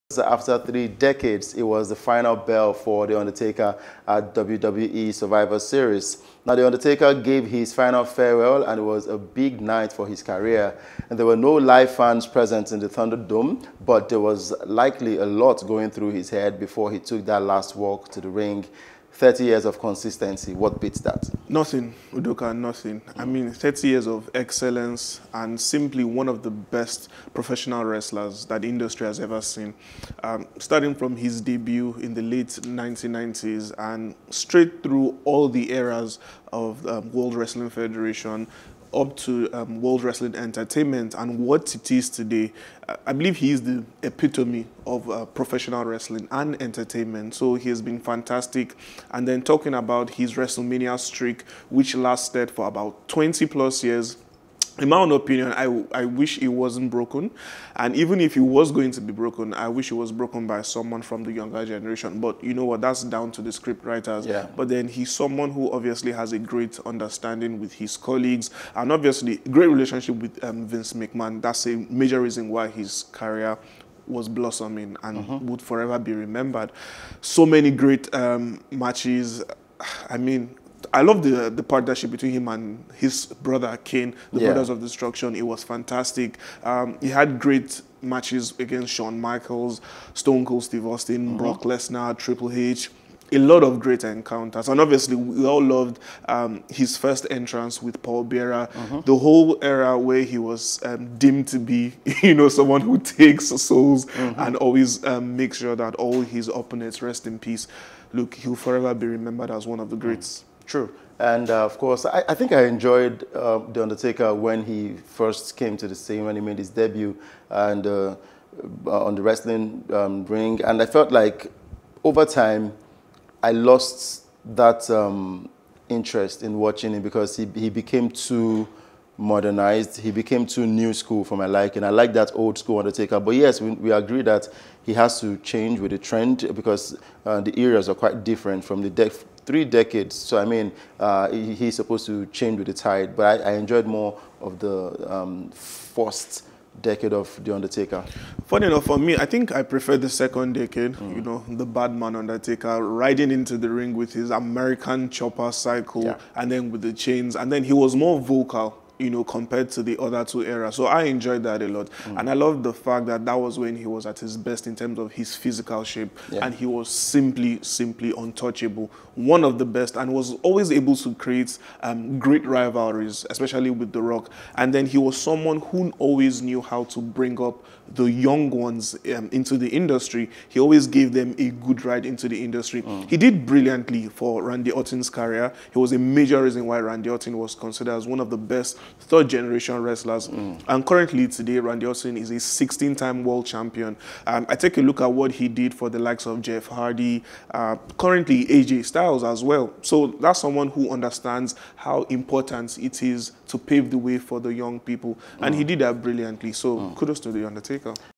The Undertaker Finally Rests In Peace
Udoka Njoku and Ope Adebari talk about the Illustrious career of now-former WWE superstar Mark Calaway popularly known as "The Undertaker"